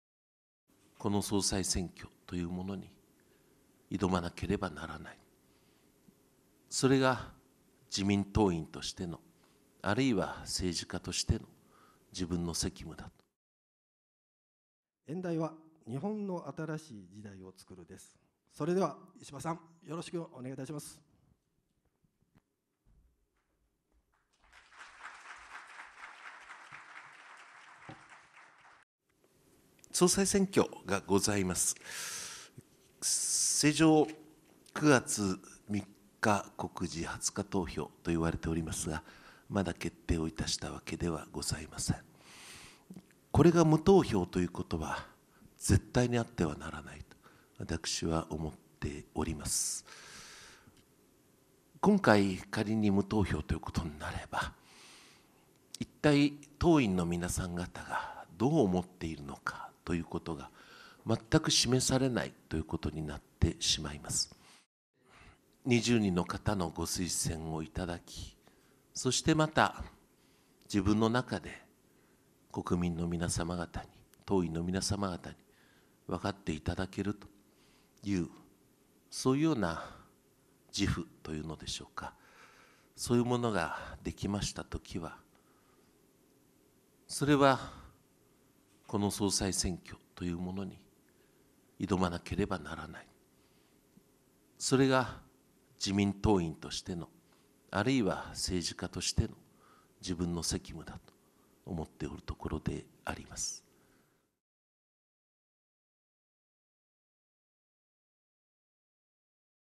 総裁選挑戦「自分の責務」 自民・石破氏が強調
自民党の石破茂元幹事長は31日、大阪市で開かれた共同通信きさらぎ会で講演し、9月の党総裁選への立候補は自身の使命だと強調した。「全身全霊をかけた準備ができれば、総裁選に挑まなければならない。それが政治家としての自分の責務だ」と述べた。